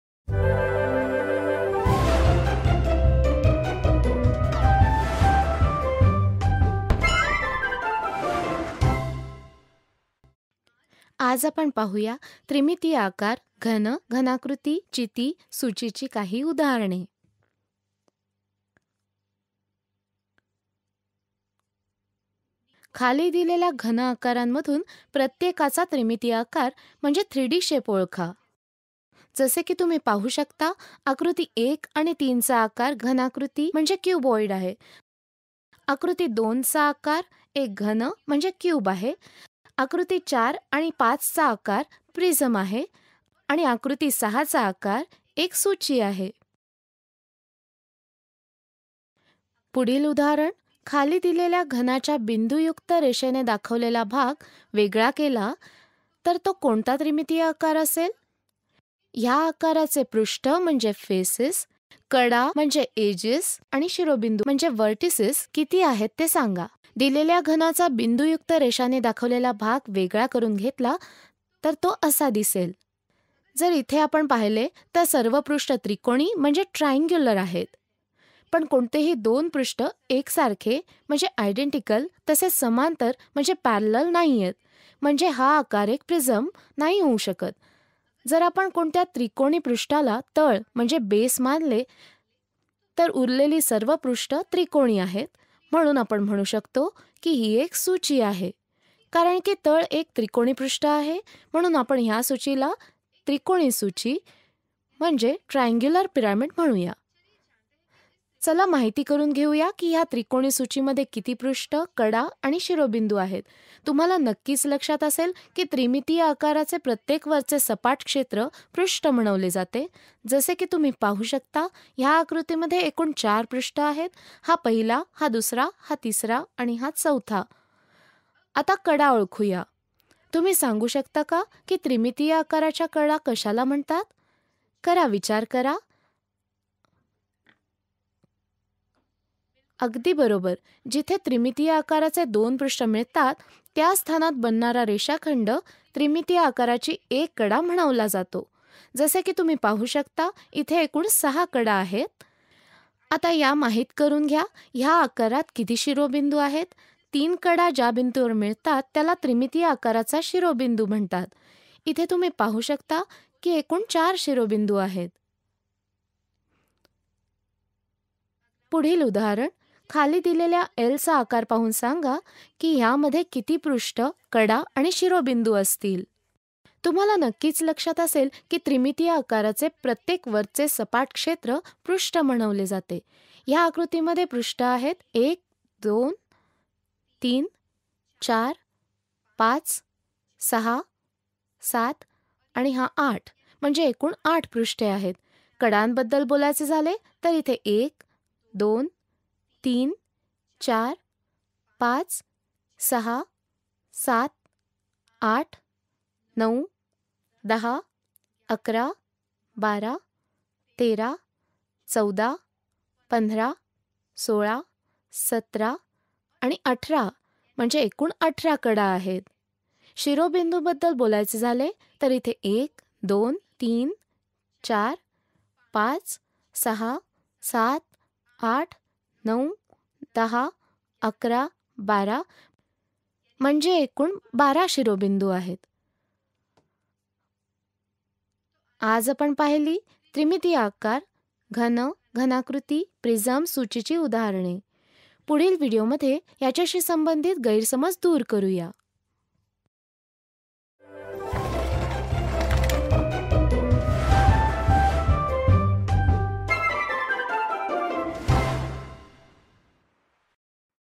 त्रिमितीय आकार|Part 2/3|Three Dimensional Shapes|Marathi|Class 6
त्रिमितीय आकार|Part 2/3|Three Dimensional Shapes|Marathi|Class 6 | आकार आणि स्थान | कक्षा 6 | NCERT | प्राथमिक आकार समजणे

Three dimensional shapes-features of cube, cuboid, pyramid, prism | Shapes and Space | Grade 6 | NCERT | Understanding elementary shapes

Welcome to part 2 of Three dimensional shapes-features of cube, cuboid, pyramid, prism. In this video, we will discuss various examples of Three dimensional shapes-features of cube, cuboid, pyramid, prism.